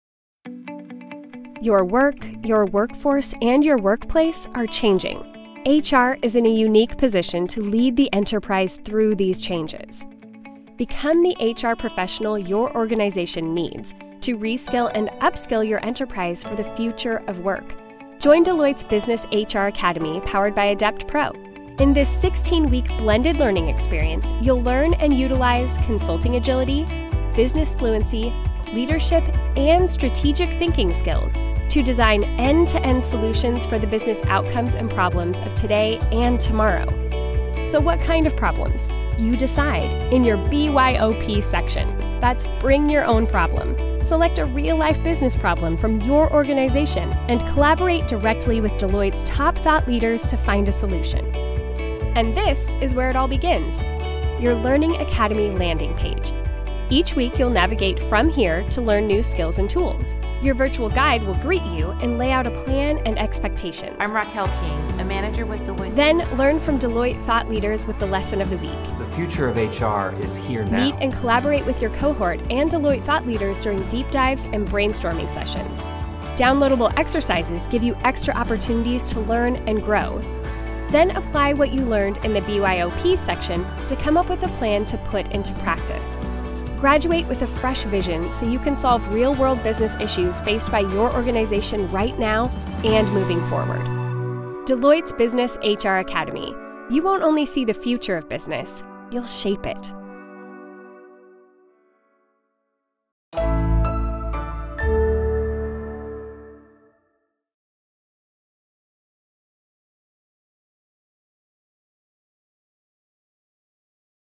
Business HR Academy Powered by AdeptPro
Positioning HR to work at the intersection of business and strategy, the Business HR Academy provides a virtual and scalable solution that brings business and HR skills and practices, insights based on research, and industry-leading thought leadership to today’s CHROs and their teams.